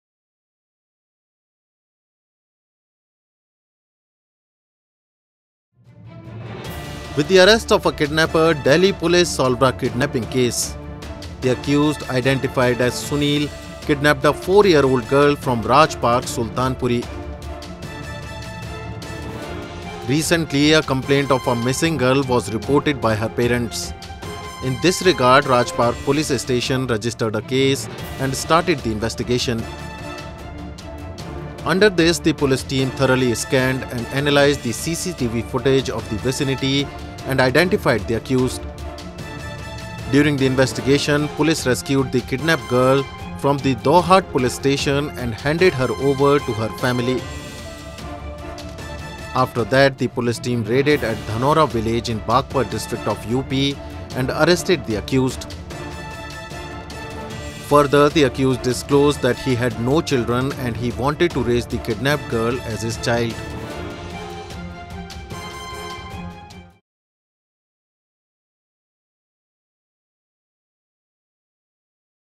Delhi Police arrests accused involved in kidnapping of girl within hours I Crime Update
New Delhi, September 12(ANI): With the arrest of a kidnapper, Delhi Police solved a kidnapping case. The accused identified as Sunil kidnapped a four-year-old girl from Raj Park, Sultanpuri. Recently, a complaint of a missing girl was reported by her parents. In this regard, Raj Park Police Station registered a case and started the investigation. Under this, the Police team thoroughly scanned and analysed the CCTV footage of the vicinity and identified the accused. During the investigation, Police rescued the kidnapped girl from the Doughat Police Station and handed her over to her family. After that, the Police team raided at Dhanaura village in Baghpat district of UP and arrested the accused. Further, the accused disclosed that he has no children and he wanted to raise the kidnapped girl as his child.